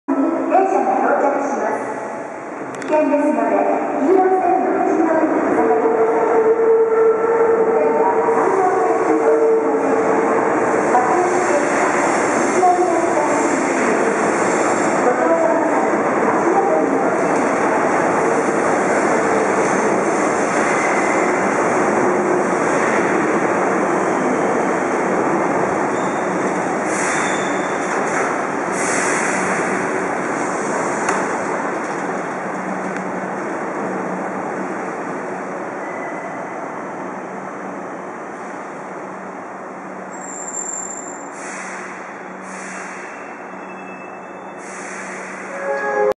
阪急宝塚駅 阪急6000系6025F+6026F 到着
Hankyu Imazu Line (North section), Hankyu Series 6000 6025F+6026F arrival at Takarazuka Station.